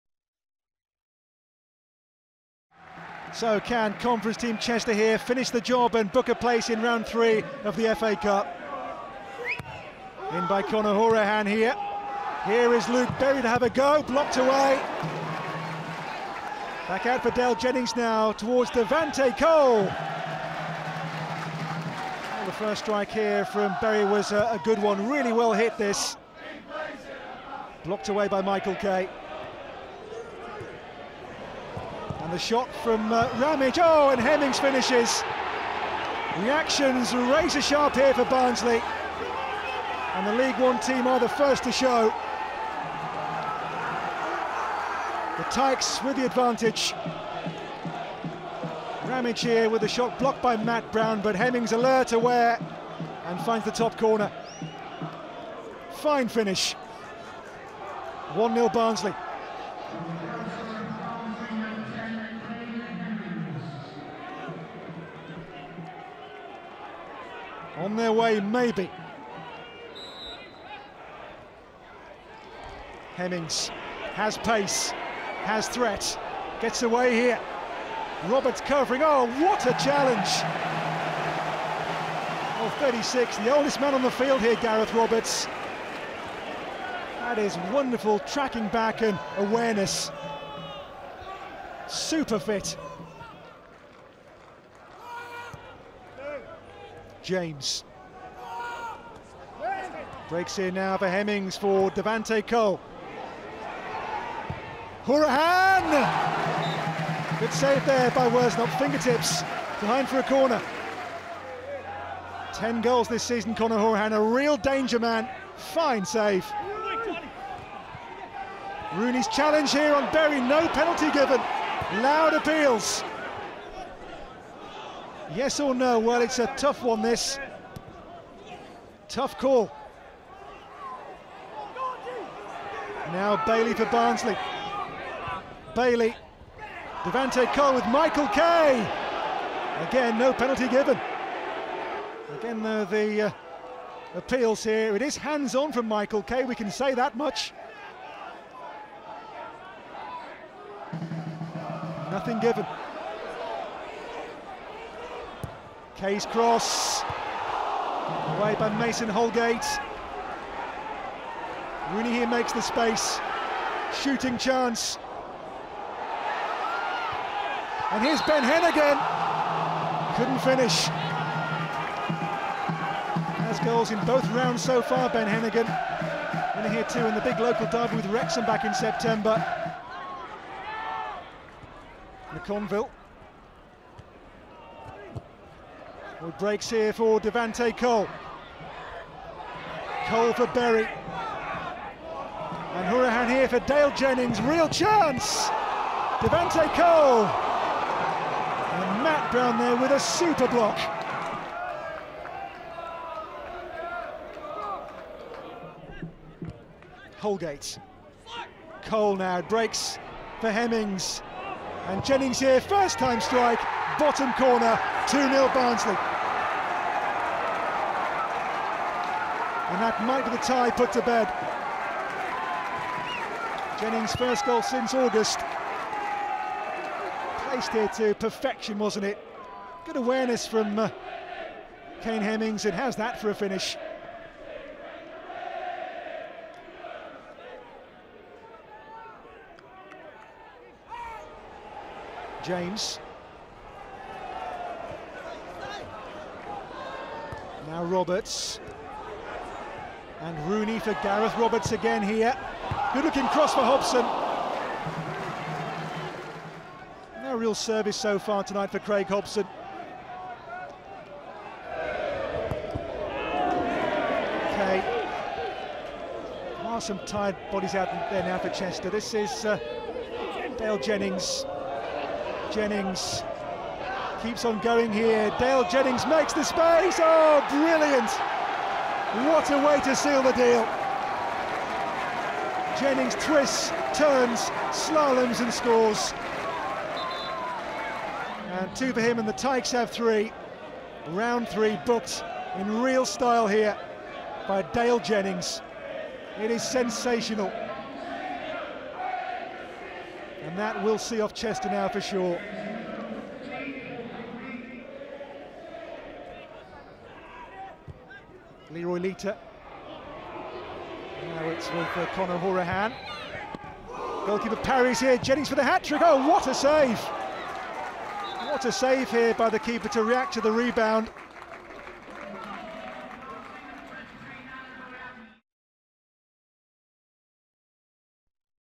Chester 0-3 Barnsley - FA Cup Second Round | Goals & Highlights
Dale Jennings struck twice as non-league Chester's hopes of another FA Cup upset were clinically ended by League One side Barnsley.
After an initial 0-0 draw, Kane Hemmings calmly stroked home from 12 yards to give the Tykes a first-half lead in the replay.